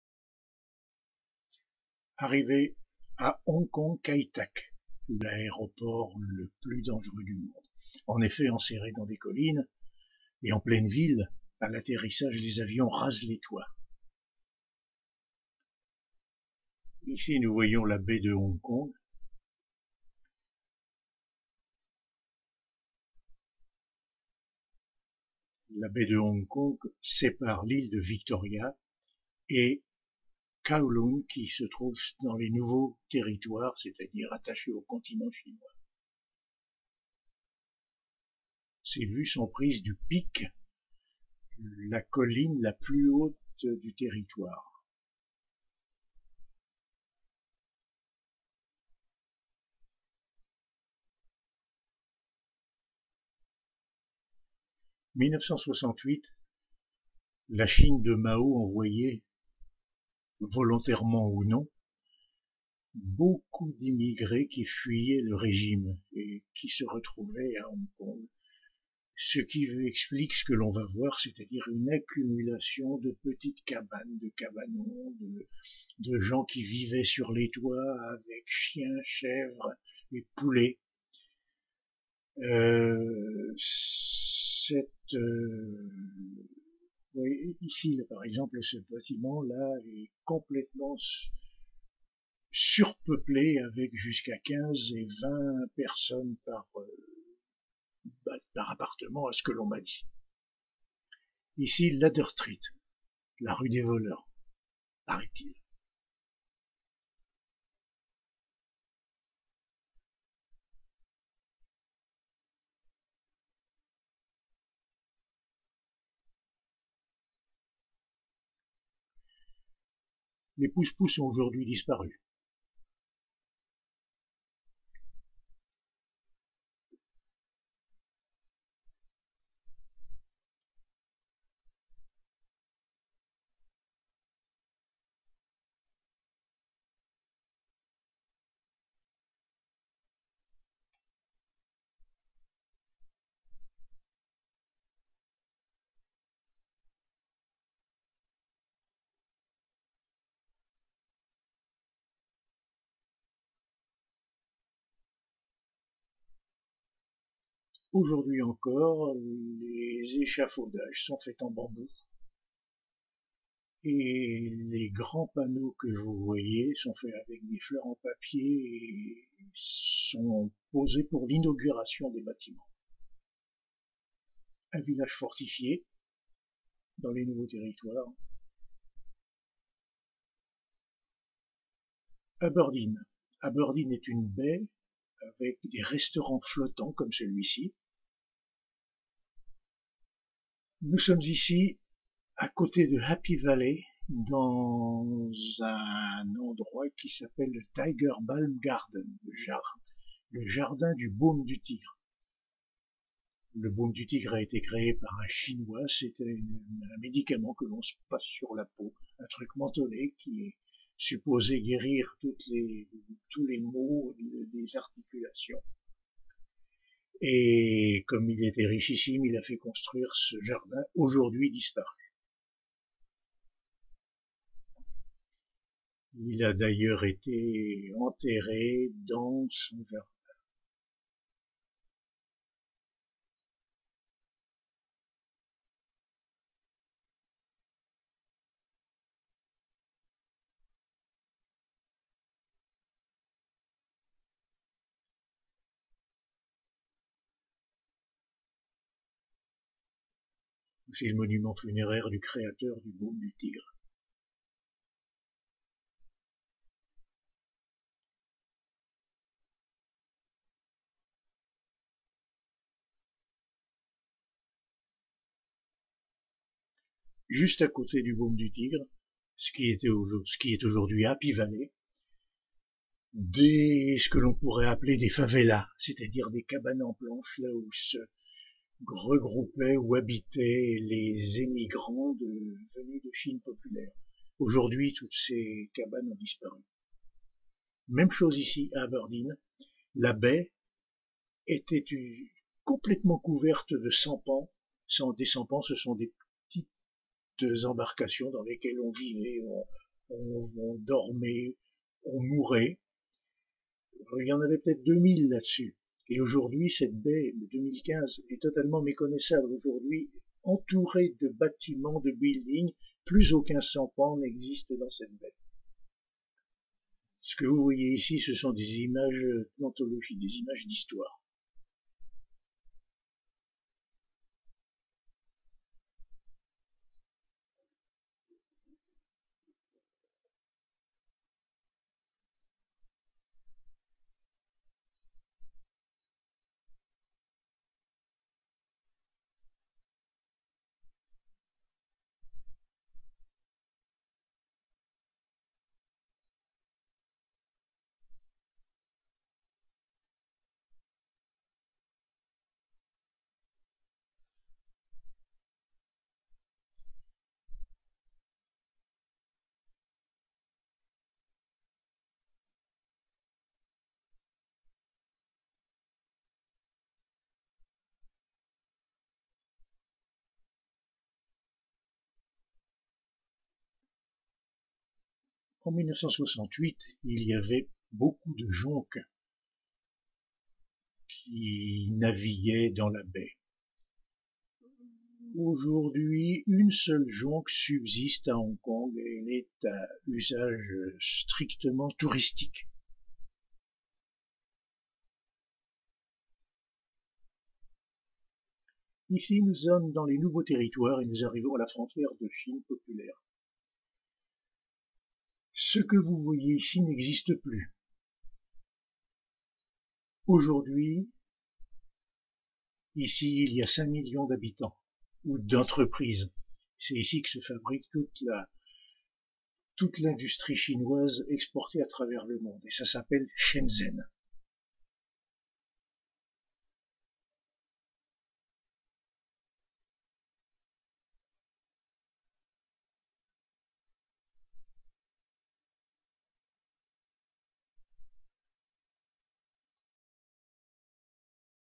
Hong Kong in 1968 (Super8)
Shot and commented by my grandfather.